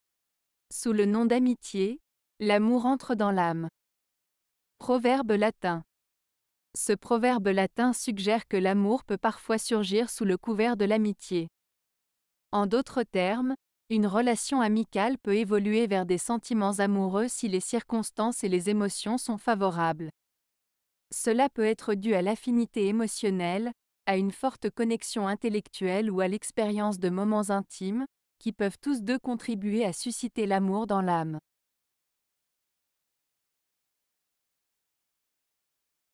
Sous le nom d'amitié, l'amour entre dans l'âme. - Proverbe Latin
Ce proverbe latin suggère que l'amour peut parfois surgir sous le couvert de l'amitié. En d'autres termes, une relation amicale peut évoluer vers des sentiments amoureux si les circonstances et les émotions sont favorables. Cela peut être dû à l'affinité émotionnelle, à une forte connexion intellectuelle ou à l'expérience de moments intimes, qui peuvent tous deux contribuer à susciter l'amour dans l'âme.